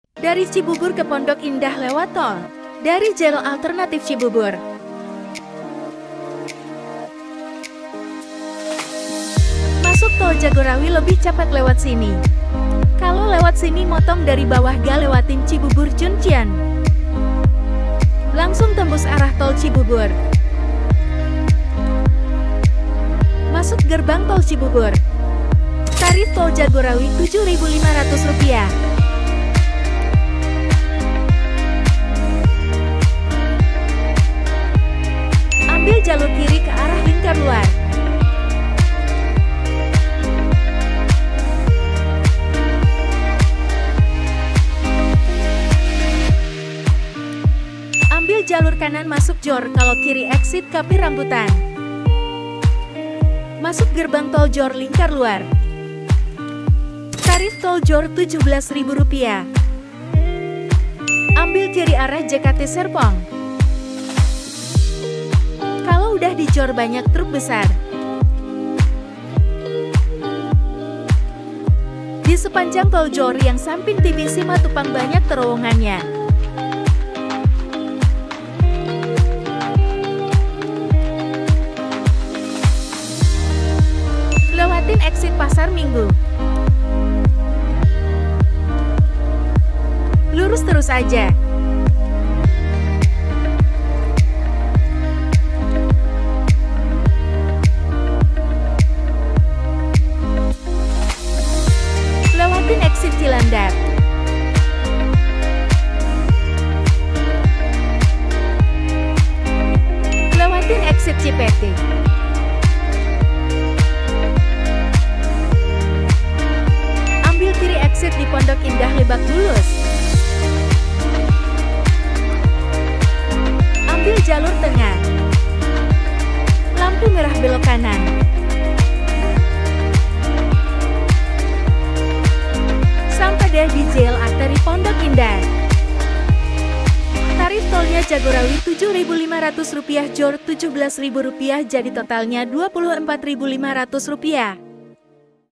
DARI CIBUBUR KE PONDOK INDAH LEWAT TOL JAGORAWI & JORR
Dari cibubur ke pondok indah lewat tol jagorawi nyambung ke tol jorr nelusurin samping jl. TB Simatupang